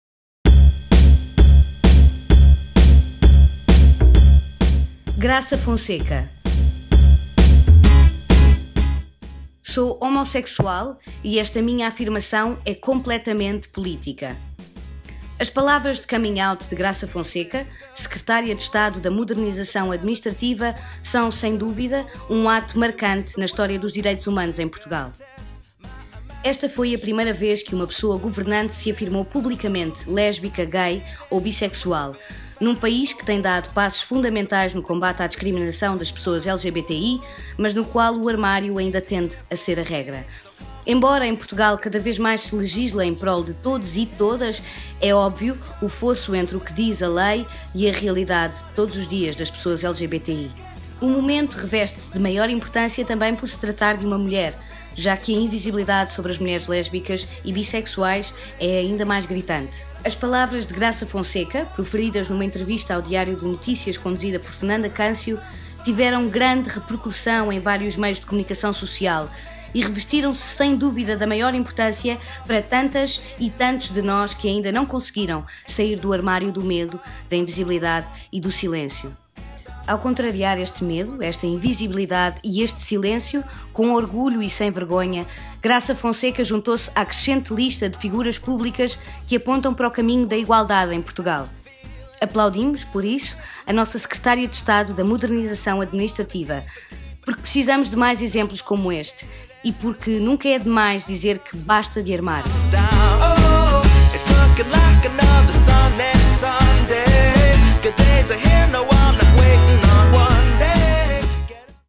Prémios Arco-Íris 2017 - Graça Fonseca
Vídeo de apresentação do prémio atribuído a Graça Fonsecam Secretária de Estado da Modernização Administrativa, por contrariar o silêncio e a invisibilidade das pessoas LGBTI ao afirmar-se publicamente como mulher lésbica - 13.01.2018 - Estúdio Time Out - Mercado da Ribeira